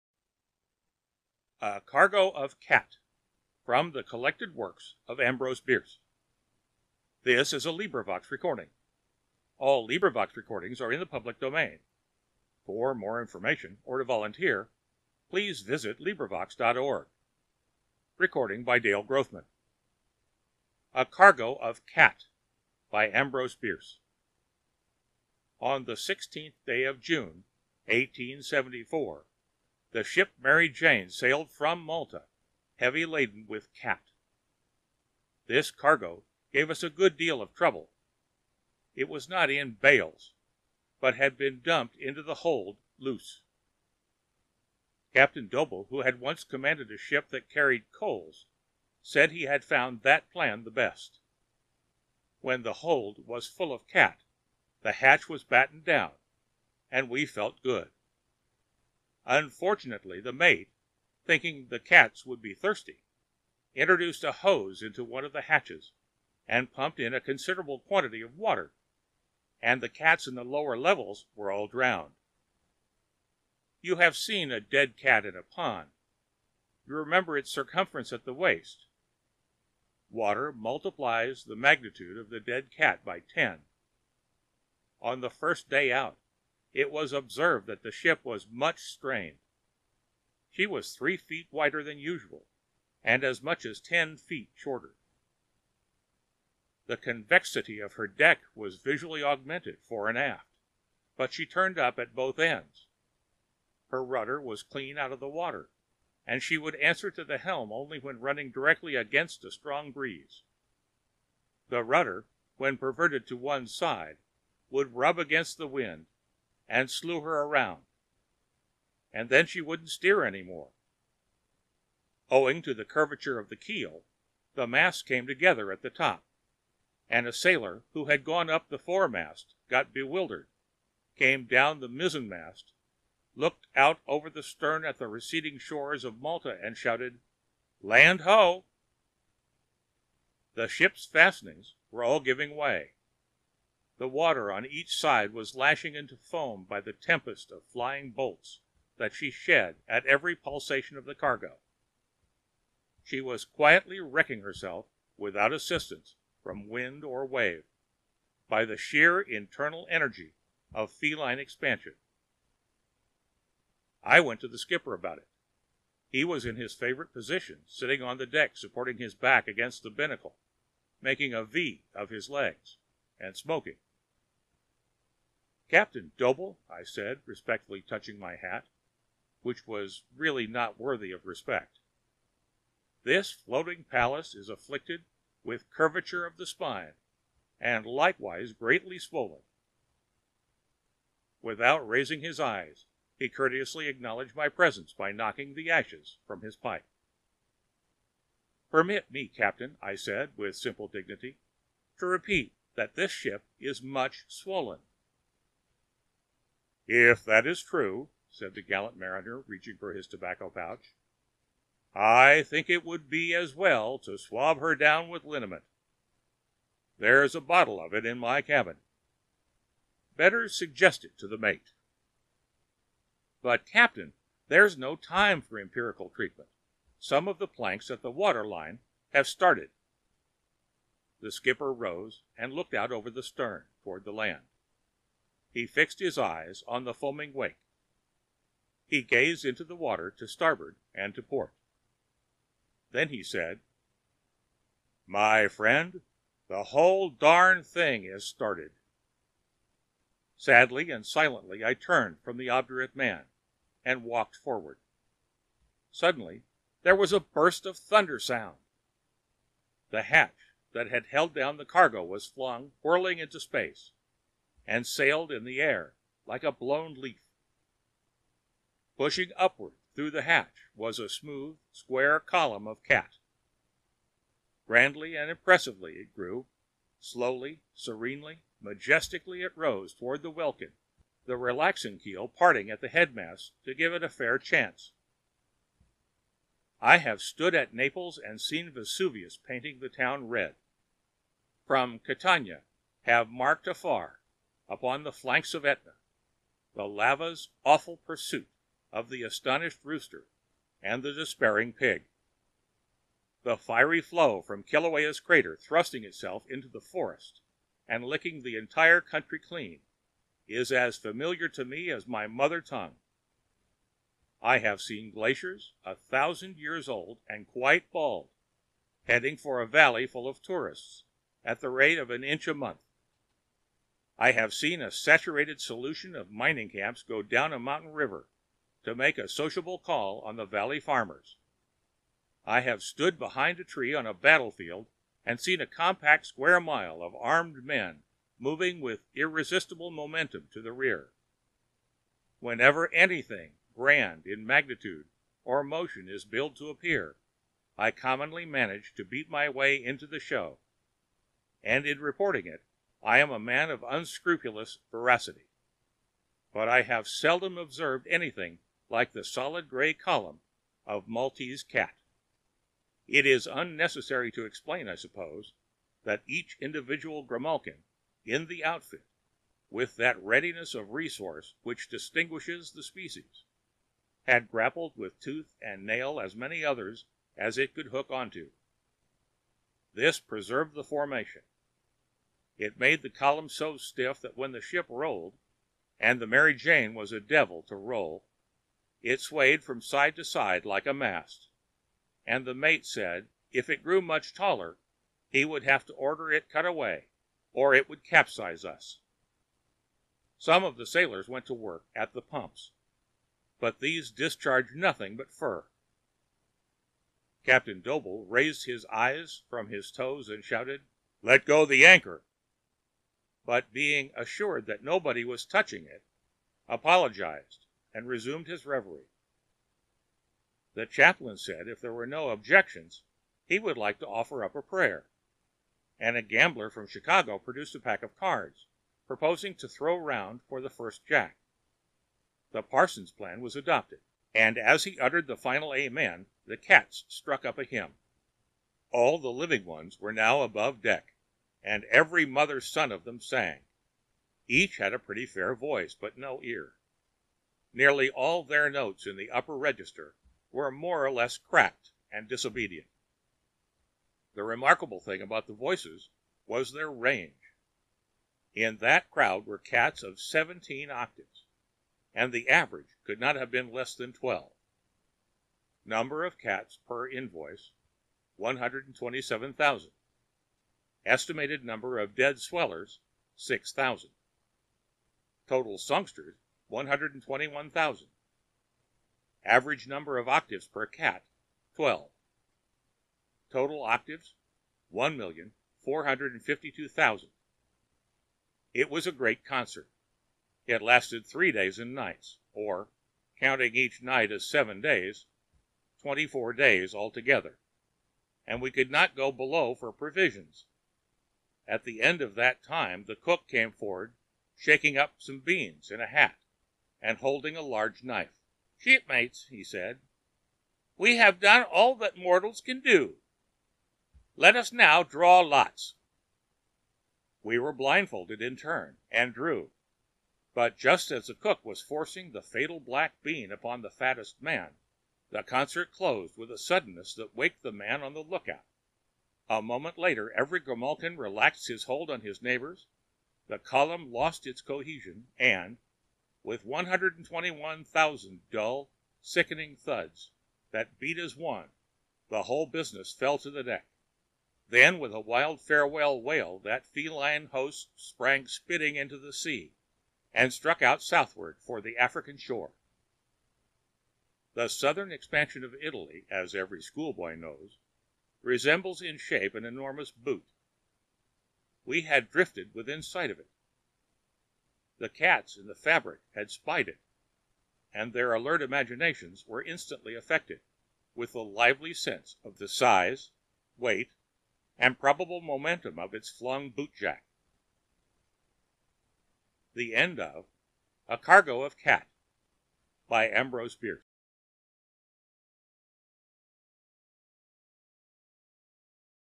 AMBROSE BIERCE "A Cargo of Cat"//Short stories
®️AMBROSE BIERCE
"A Cargo of Cat"®️

First published as by "B," in the Wasp (San Francisco), January 3, 1885

"After his ship springs a leak during a voyage, a sailor details the amusingly bizarre dangers that one can face when transporting a cargo of cats across the open seas."